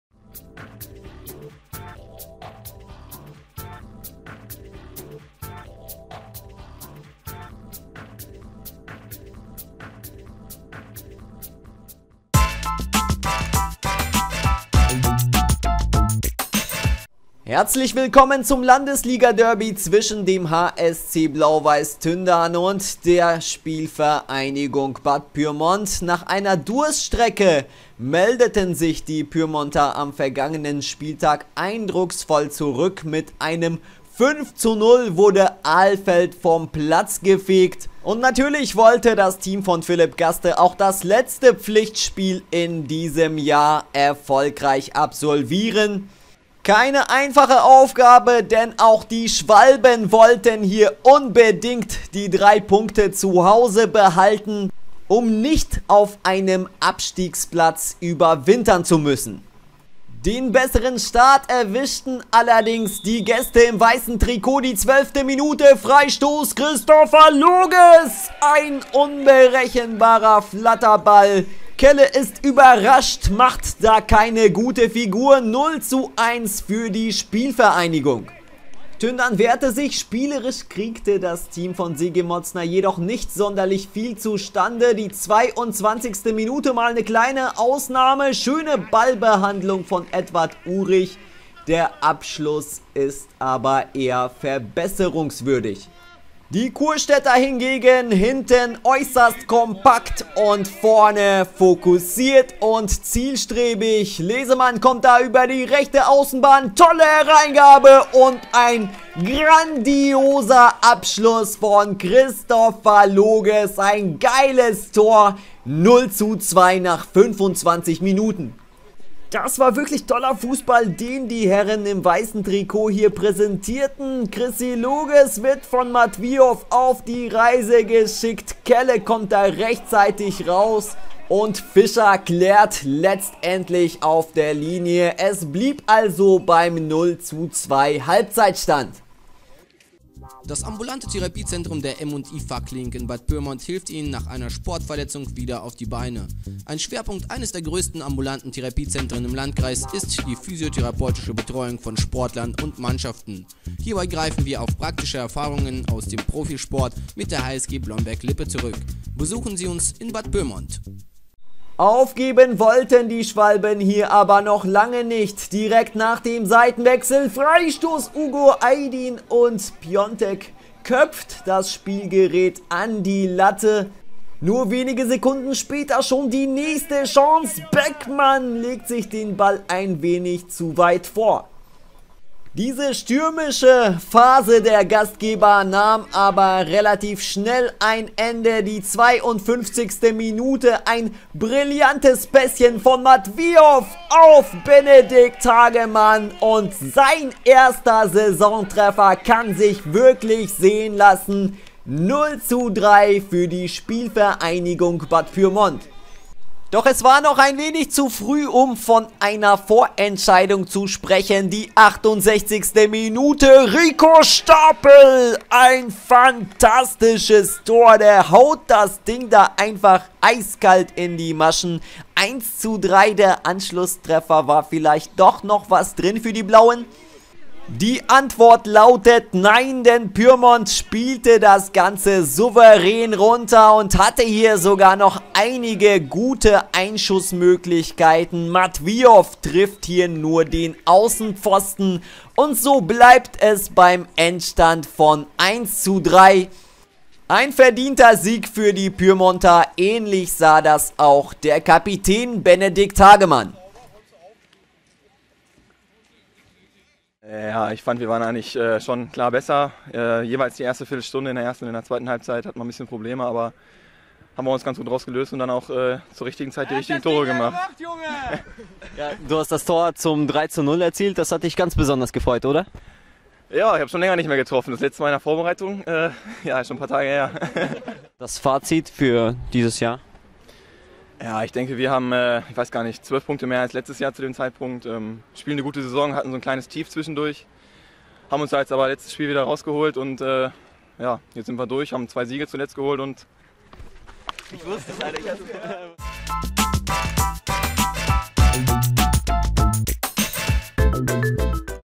HSC BW Tündern - SpVgg Bad Pyrmont
Wer dieses Landesliga-Derby für sich entscheiden konnte, erfahren Sie hier.